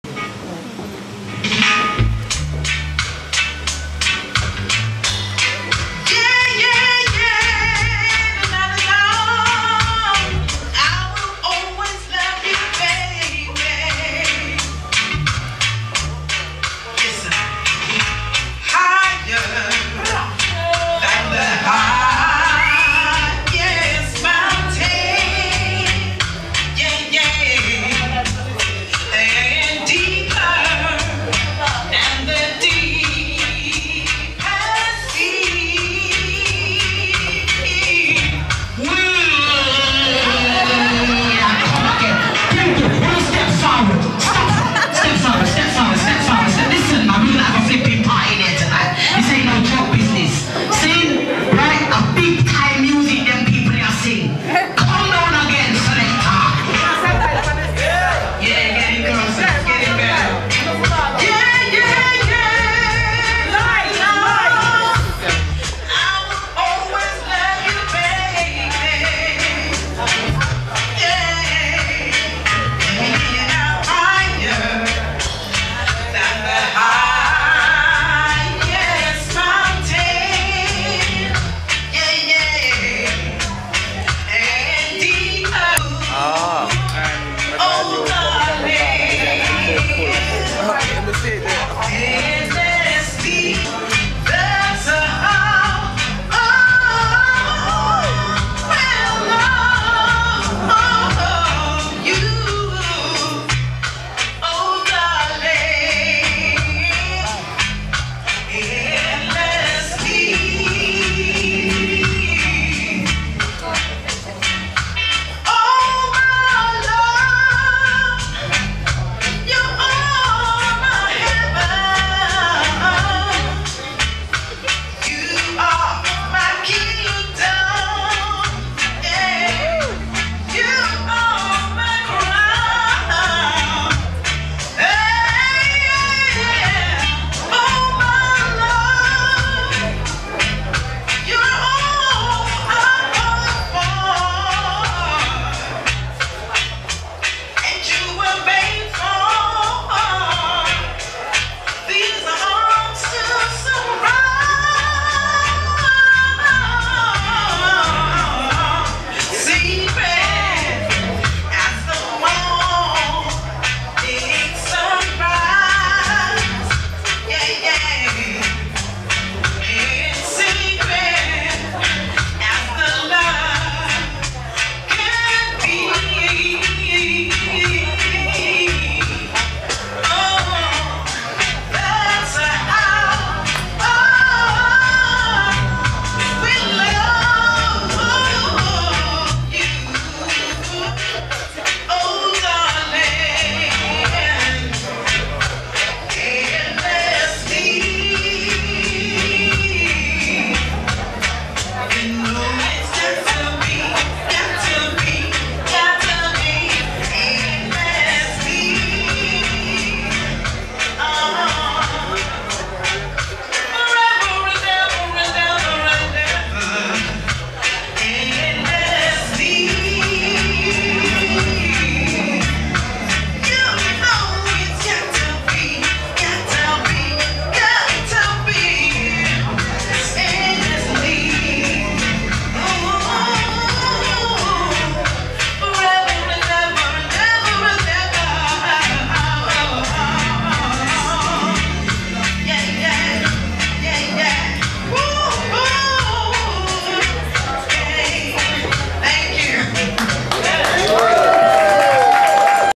Lorna G and Friends @ Scandals 21Jun14 Pt 1 (Donna Marie)
Lorna Gee presents Lorna Gee & friends  at the  Scandals Club, West Norwood on 21st June 2014 Part 1. Donna Marie.